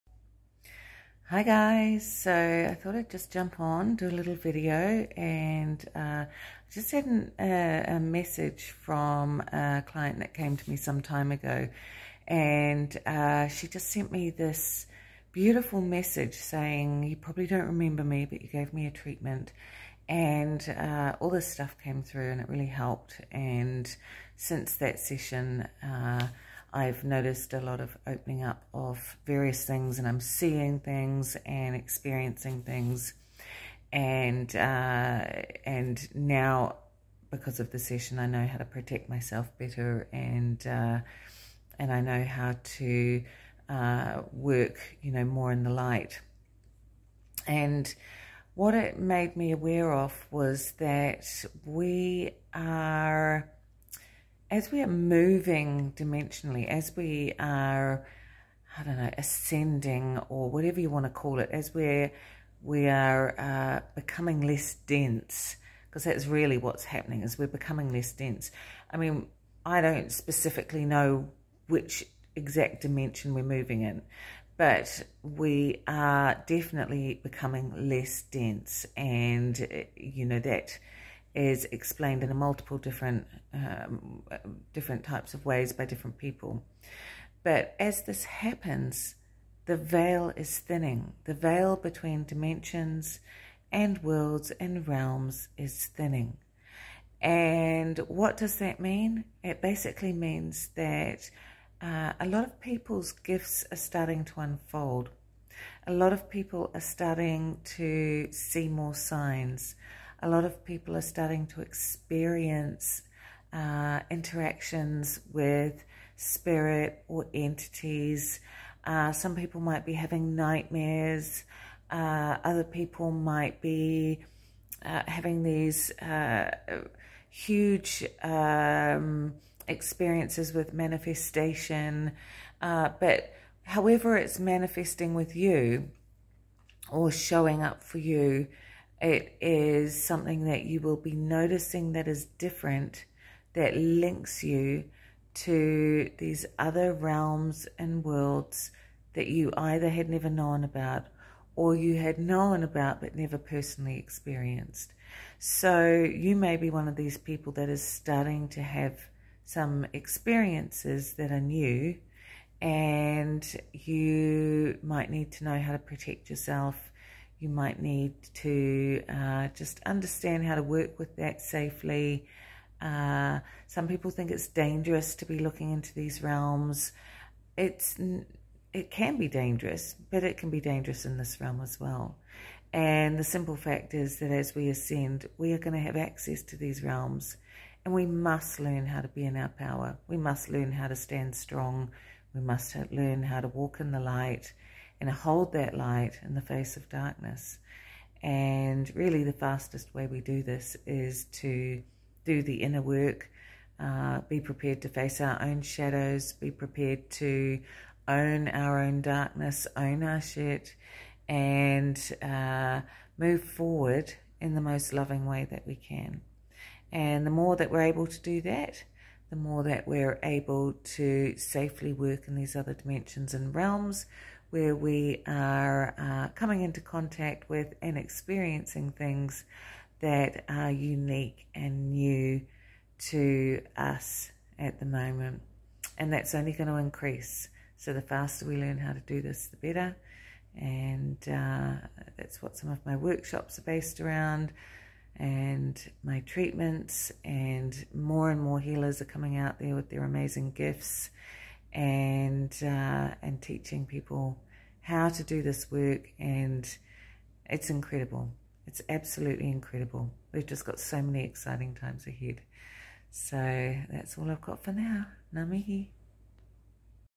Energy update 8 may 2023
Energy update 8 may 2023. The veil is thinning and we are beginning to experience new and exciting things!

As we become less dense in vibration, we begin to access other dimensions and realms with more ease so make sure you know how to walk in the light with prayer and protection 💜💜💜